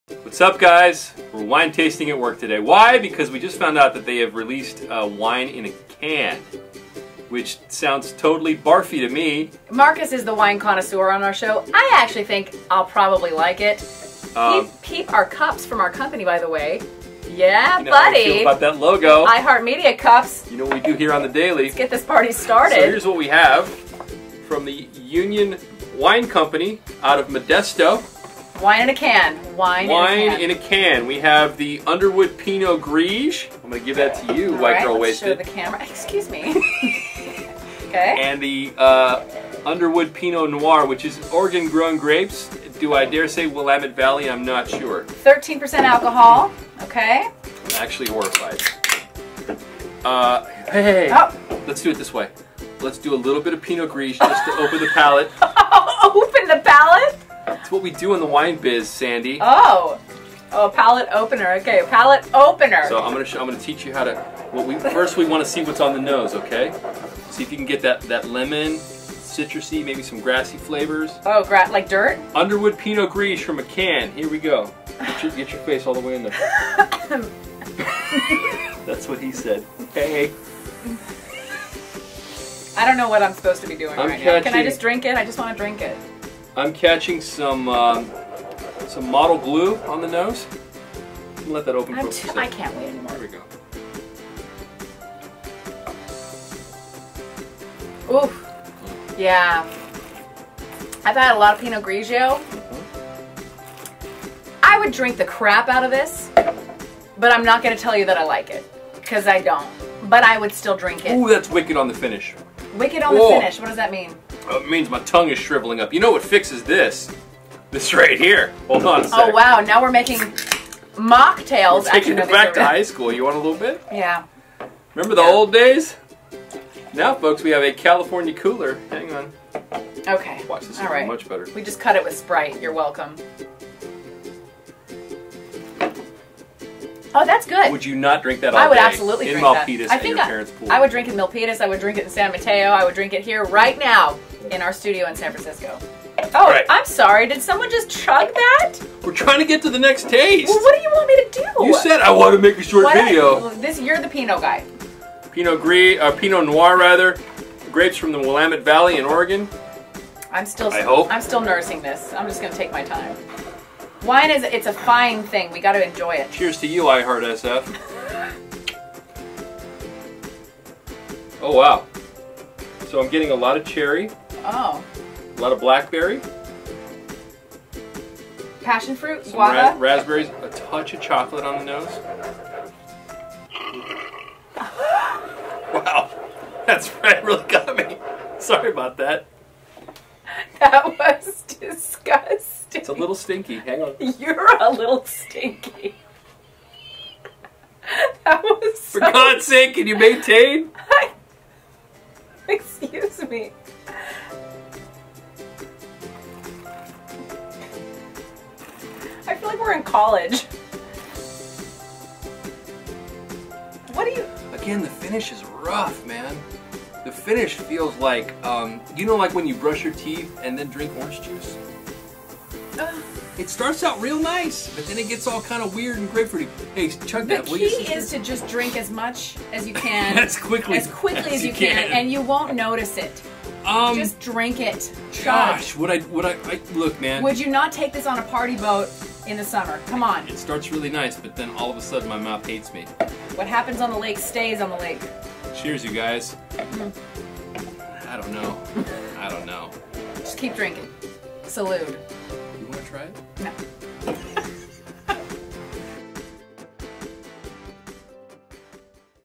WINE FROM A CAN?? - Marcus & Sandy Try
On this new show Marcus & Sandy try and review new trends! On today's episode: Underwood canned wine from the Union Wine Company.